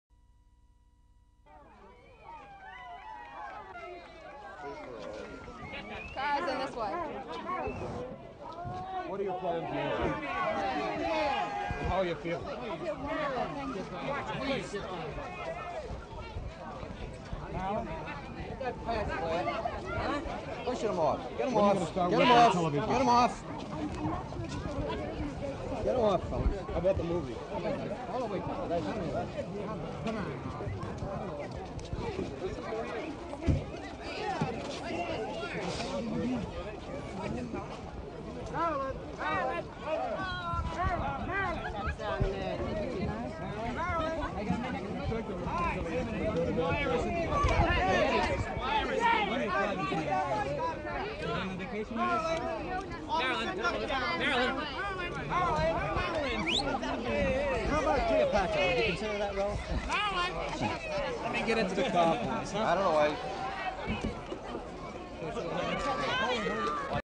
Rare Footage Marilyn Monroe of leaving Columbia Presbyterian Hospital March 5, 1961
Watch Marilyn's reaction to the reporter when about "Cleopatra" was curious and she was speechless because it was tasteless. Elizabeth Taylor was currently in a London hospital fighting for her life, shutting down the first production of "Cleopatra".  NOtice how Pat Newcomb is holding a newspaper with Liz on the front page. While MM was hospitalized at Columbia a rumor circulated that Liz had died. A very upset MM called John Springer, who was both their NY press rep.  He calmed her down. Marilyn and Liz were in competition and one was fairing better at the box offices, but still knowing MM character she never would have wished ill on Liz. ALLABOUTMARILYN.COM